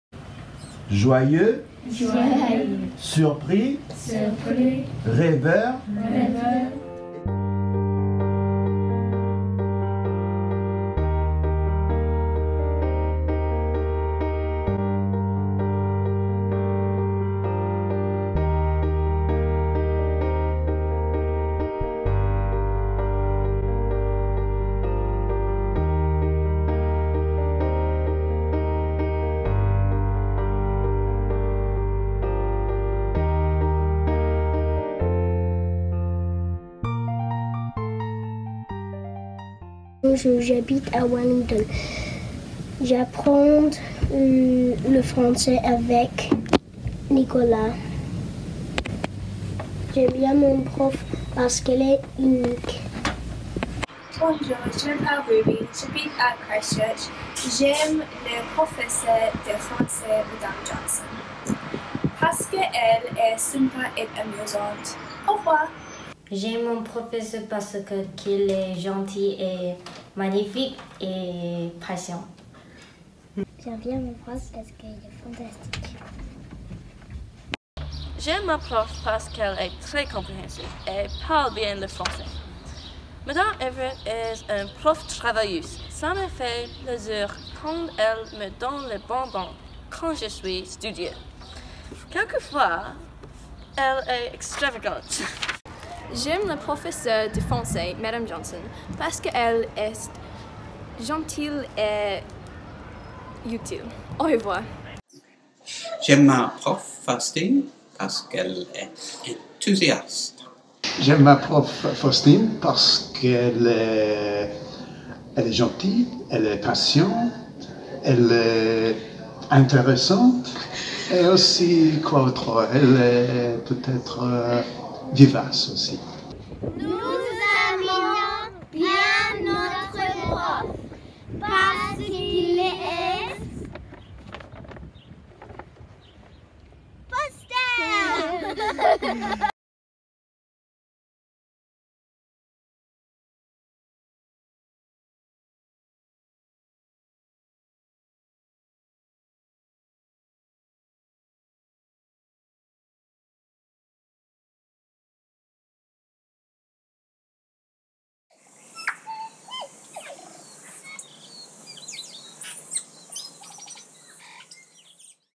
French teachers in New Zealand
On November 28, we celebrate the French teachers day all around the world.
Come and meet some of the 45 000 students learning French in New Zealand. They will explain you why they like their French teachers!

Le 28 novembre, nous célébrons la journée des profs de français !
Venez rencontrer quelques un(e)s des 45 000 élèves apprenant le français en Nouvelle-Zélande. Ils vont vous expliquer pourquoi ils aiment leurs profs de français !